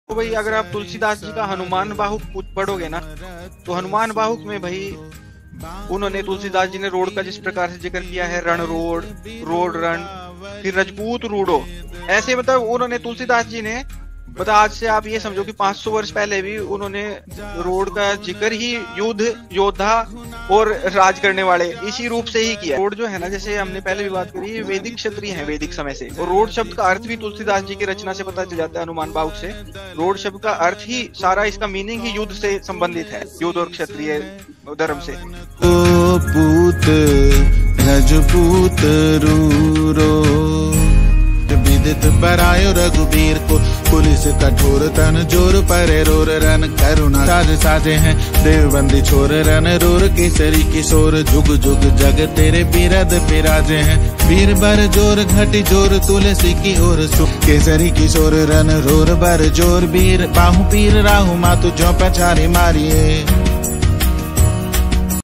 रोड़ जाति का स्वर्णिम इतिहास। Ror Caste History | Vedic Arya जाति रोड़ | रुड़, रोड़= ऊँचे कुल का/ योद्धा
रुड़, रोड़= ऊँचे कुल का/ योद्धा
रोरुका मतलब योद्धाओ की नगरी

तुलसीदास जी ने आज से 500 साल पहले हनुमान बाहुक पुस्तक में रोड़ शब्द का अर्थ योद्धा, राज करने वाले लिखा हुआ है । रोड़ समाज प्राचीन समय से प्रभुत्वशाली रहा हैं । ऐसे ही नहीं रोड़ों को रोड़ राजे कहा जाता !!
In frame: - Dr. Anurag Kadian
Reference: Hanuman Bahuk by Tulsidas ji